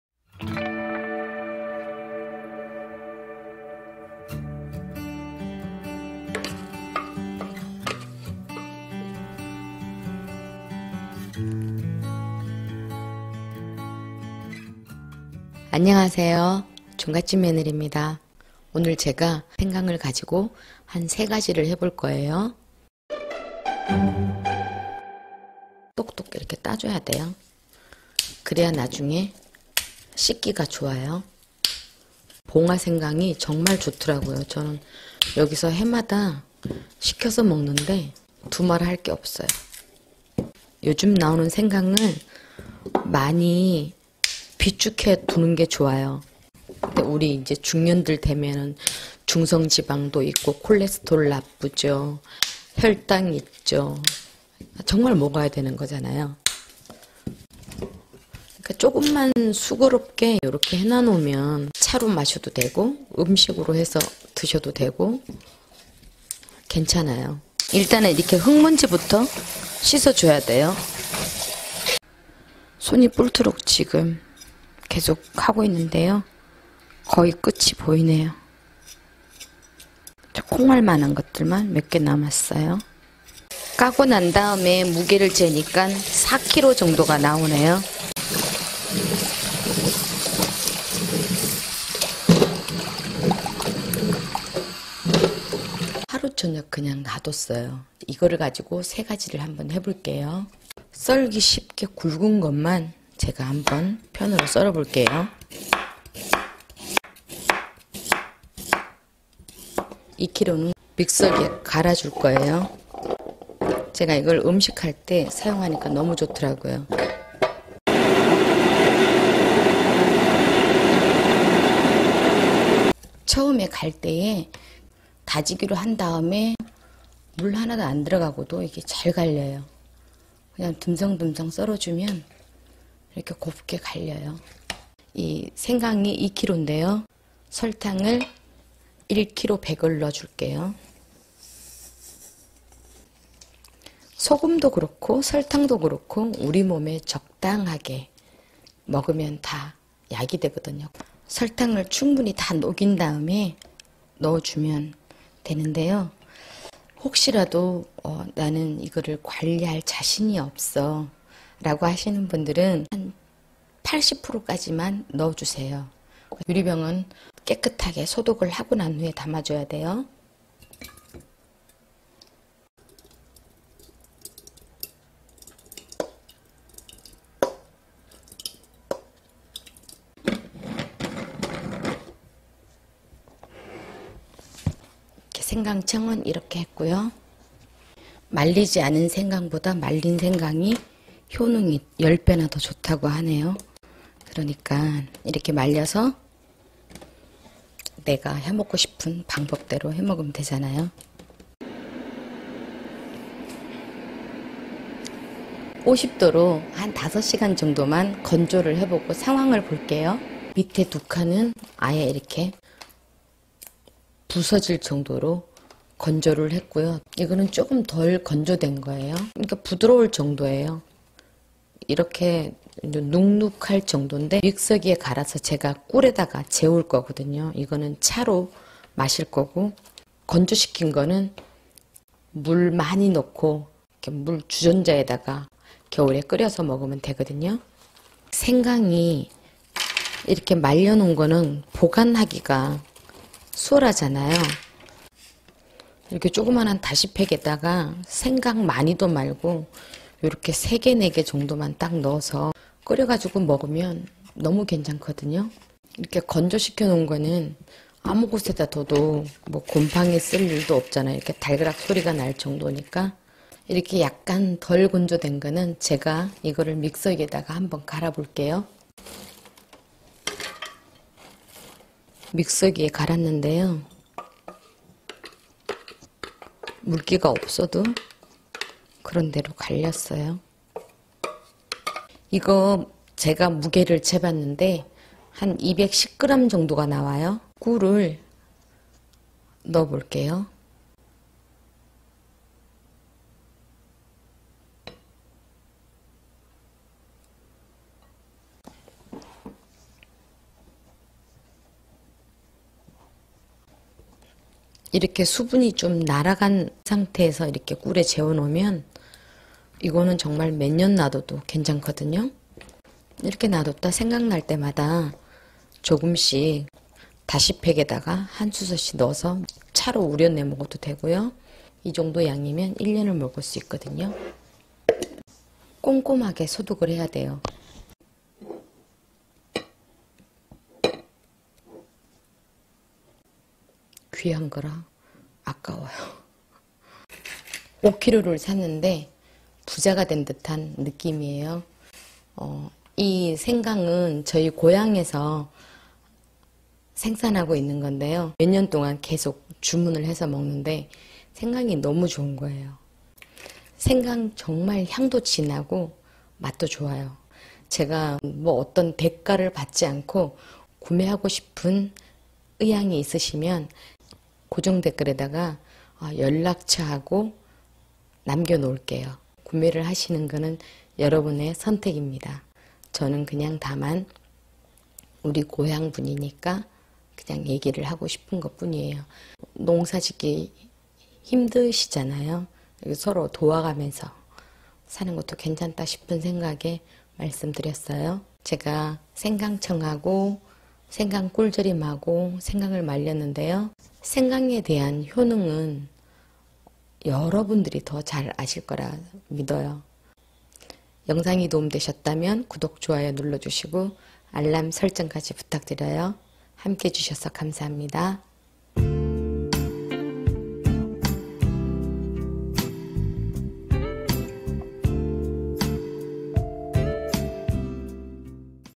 [생강효능] 생강청 만들기 말린 생강 꿀절임 생강차 꼭 드세요! #123
안녕하세요
종갓집 며느리입니다.
생강이 몸에 좋다는건 모두가 알고 있지만
어떻게 먹는게 효과적일까요?
생강을 말리면 효능은 10배나 높아진다고해요.
생강차로 마시면 성인병 예방에 좋은거 아시죠?
오늘 3가지를 만들었어요

생강 주문은 2021년도에도 마감되었습니다
성원 해주신 모든 분들 진심으로 감사합니다

생강으로 만든 3가지
만드는 재료

생강청
생강 2kg
설탕 1.100g
(요리할때도 좋지만
차로 우려 마셔도 좋아요.)

생강 꿀절임
1kg를 반건조한
생강 210g
꿀 500g
(끓인 물에1스푼씩 차로 우려 마신다.)

생강차
1kg의 생강을 건조해서
용기에 담아 보관한다
( 주전자에 생강 3~5개와 감초를 넣고
끓여서 마신다)

만드는 과정은 영상을 참고해 주세요